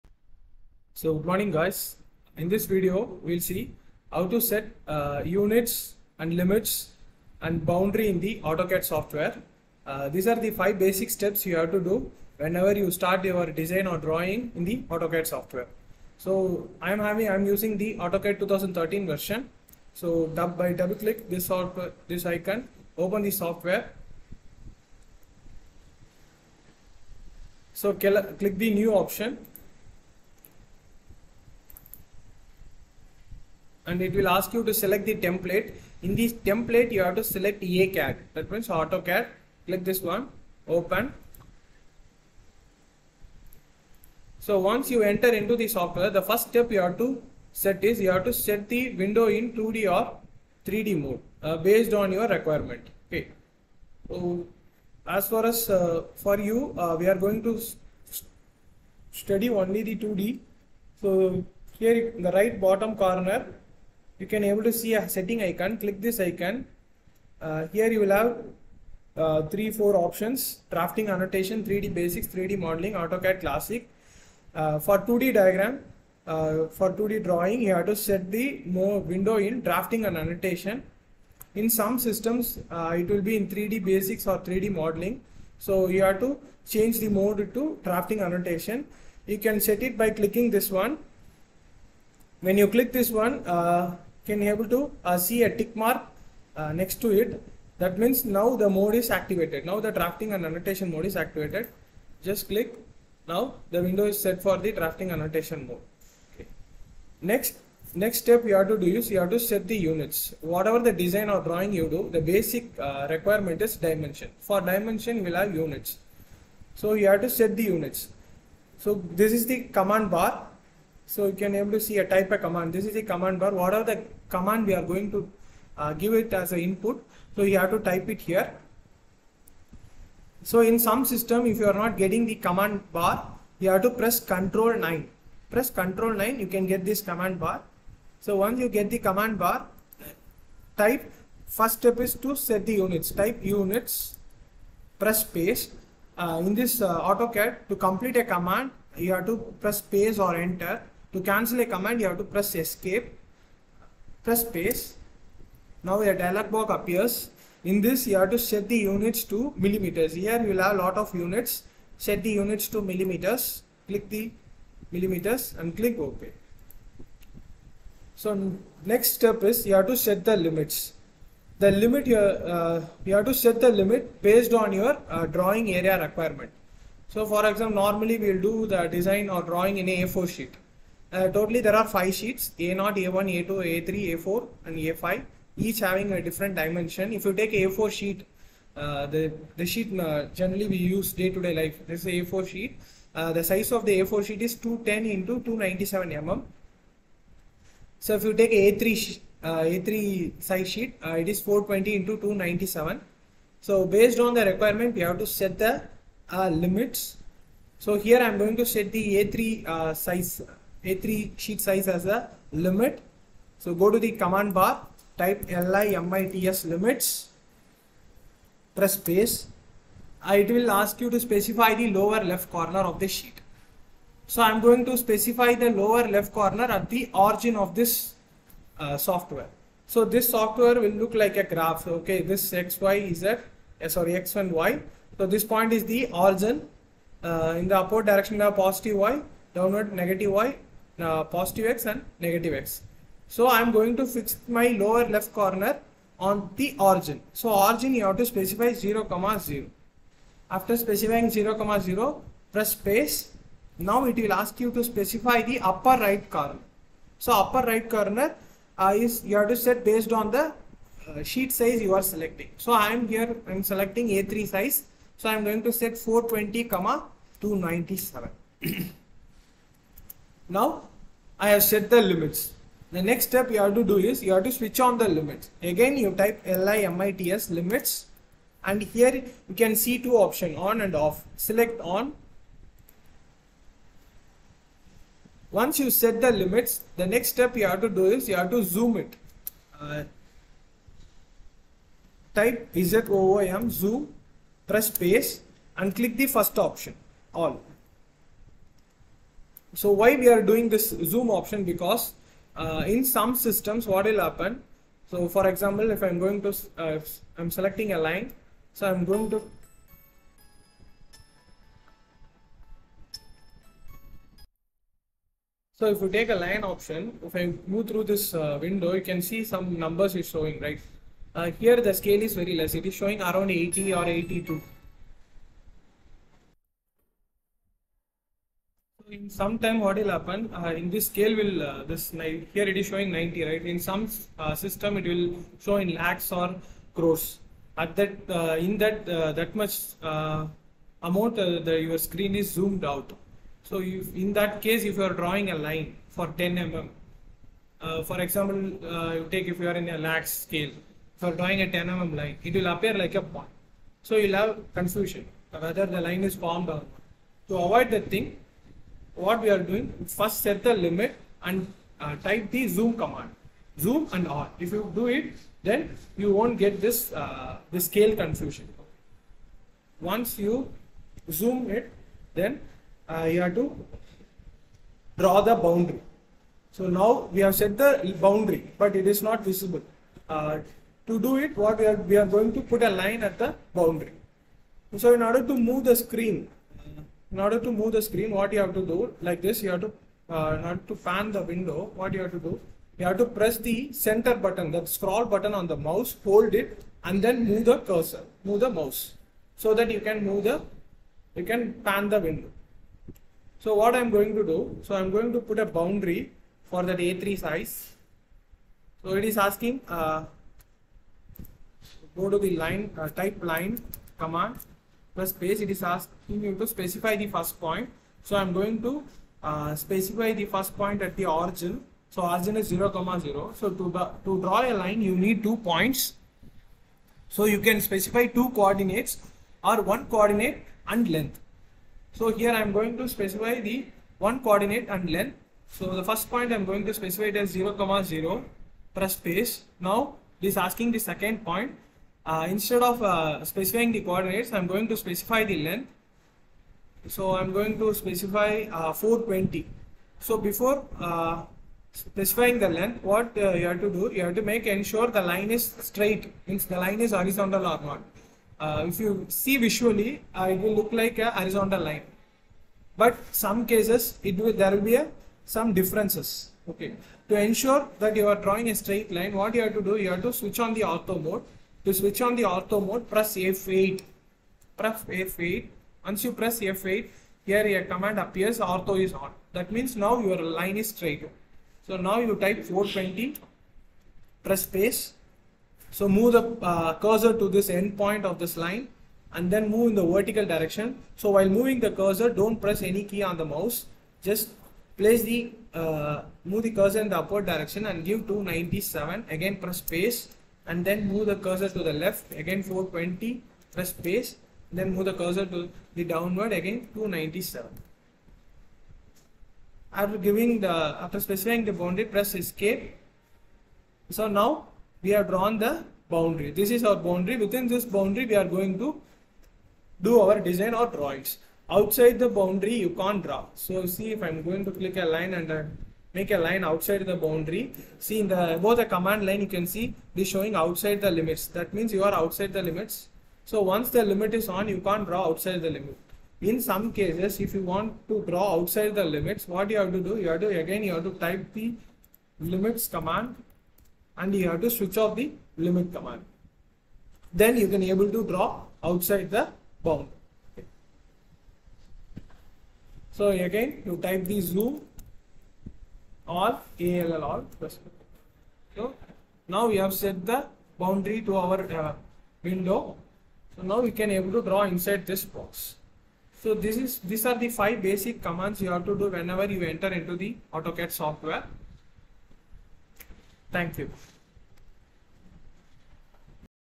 How to set units, limits and boundary in AutoCAD software! English
This video explains how to set drafting mode, units and limits in AutoCAD software.